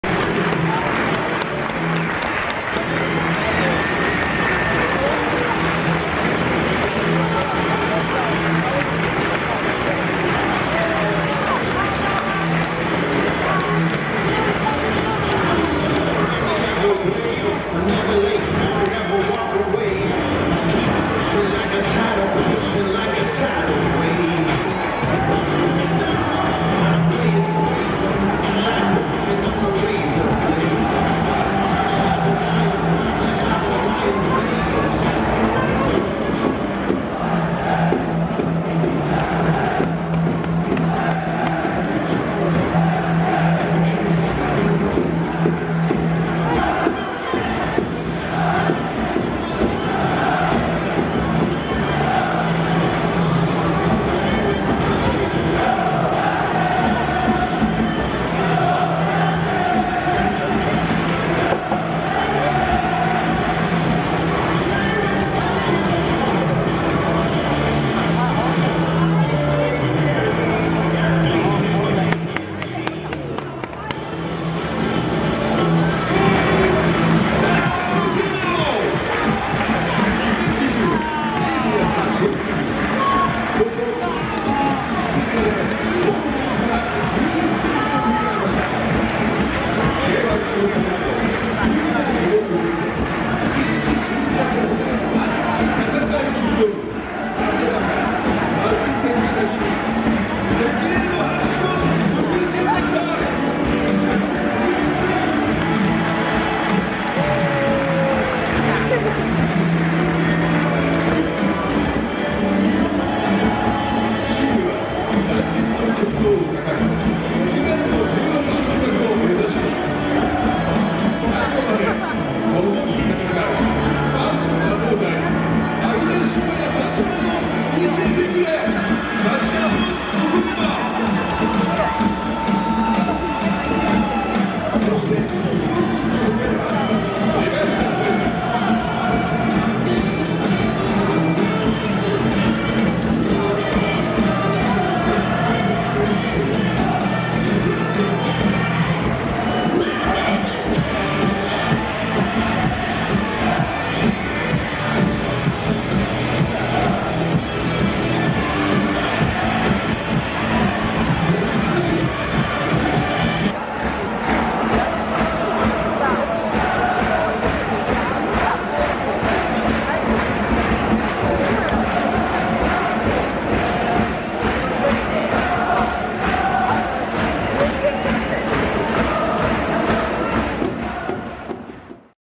【2024 岡山vs仙台】岡山 ウォーミングアップ前 ゴール裏
J2第23節 ファジアーノ岡山 / ベガルタ仙台 / シティライトスタジアム(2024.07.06)
FAGIANO OKAYAMA